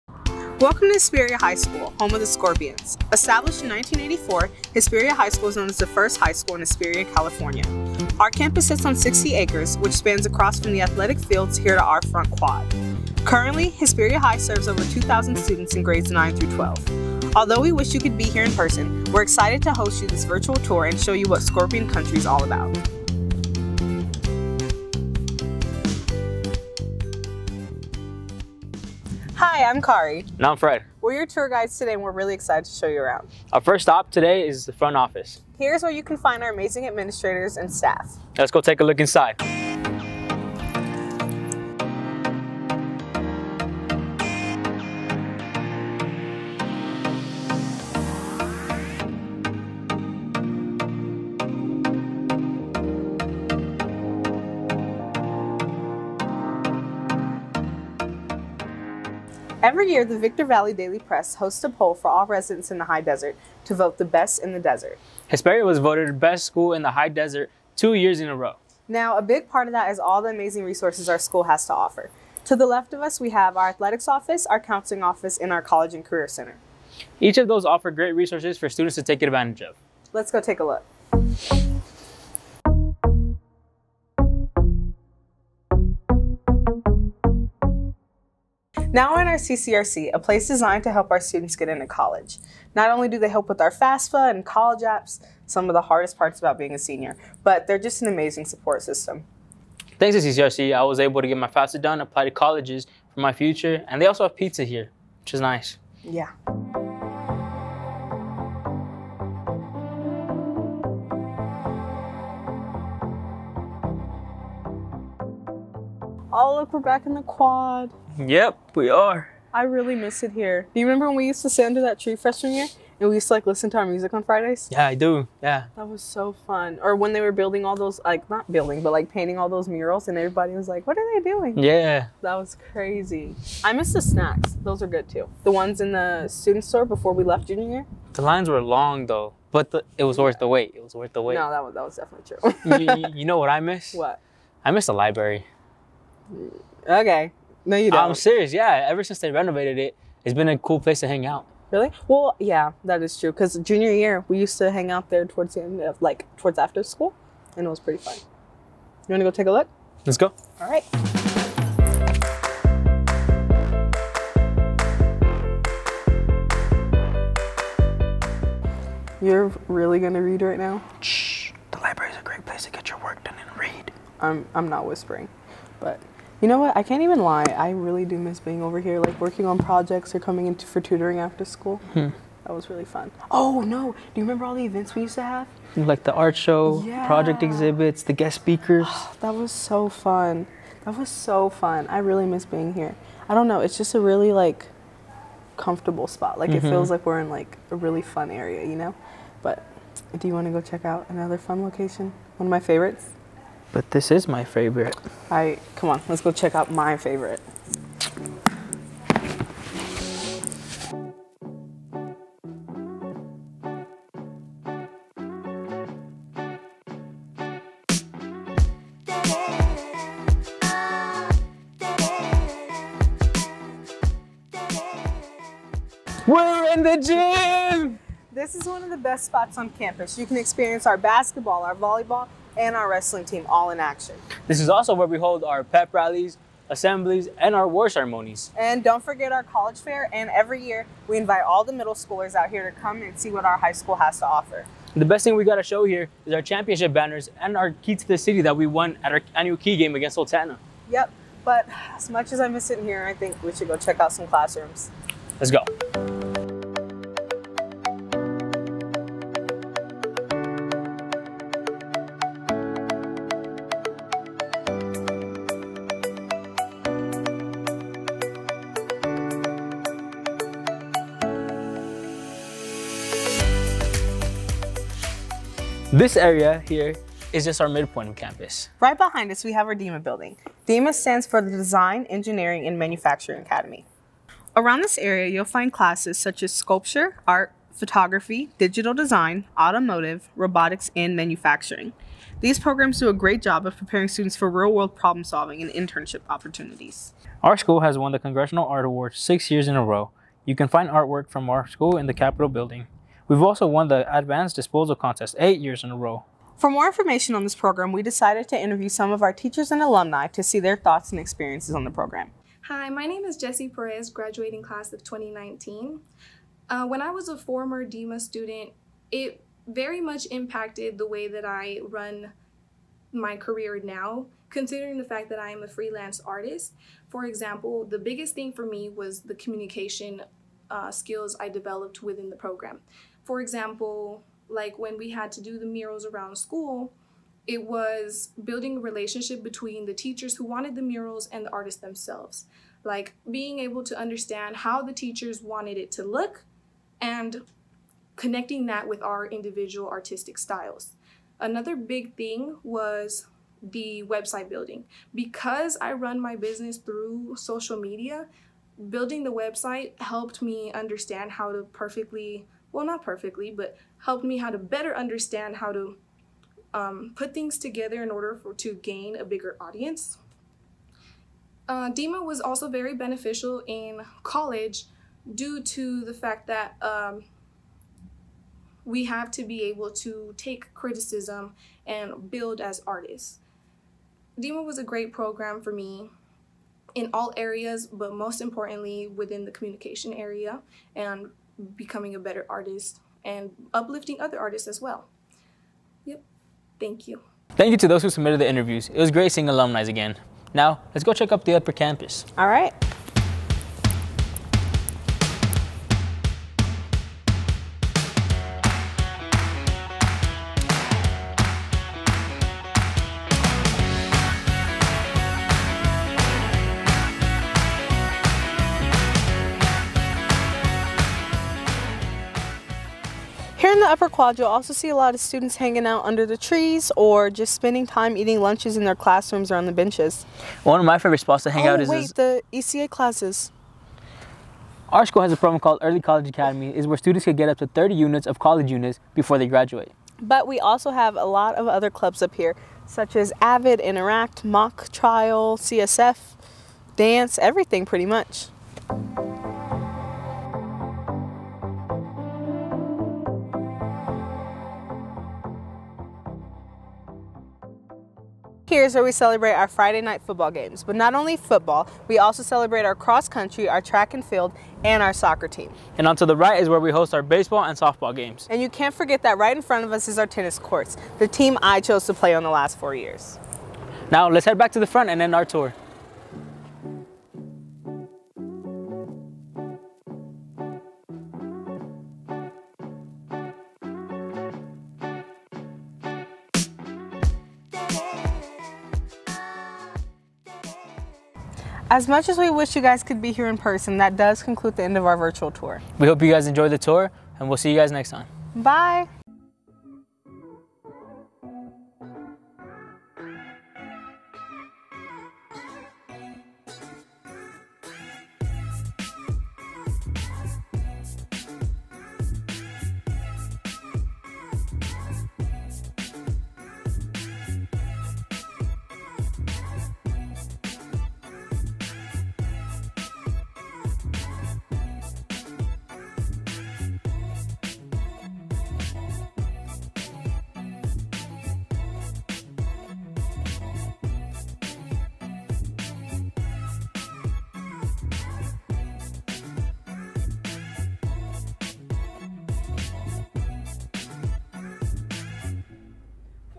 Virtual Tour of Hesperia High School
A virtual tour of Hesperia High School located in Hesperia, California.